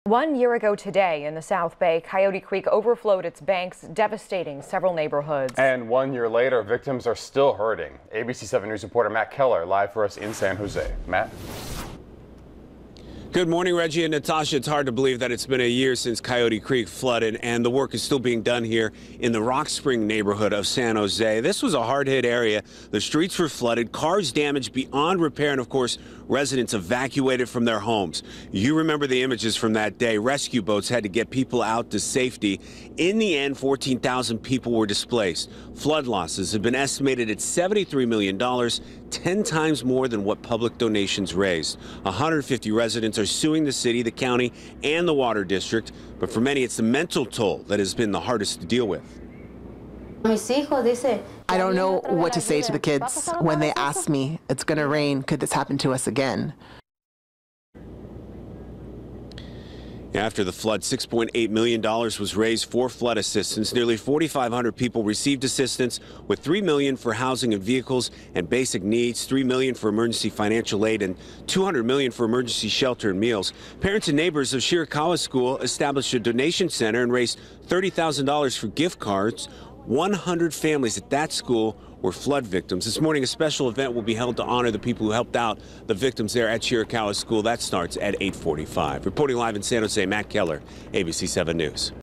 San Jose flood victims mark one-year anniversary since disaster
San Jose flood victims are marking the one-year anniversary today since the Coyote Creek disaster, which left thousands of residents displaced.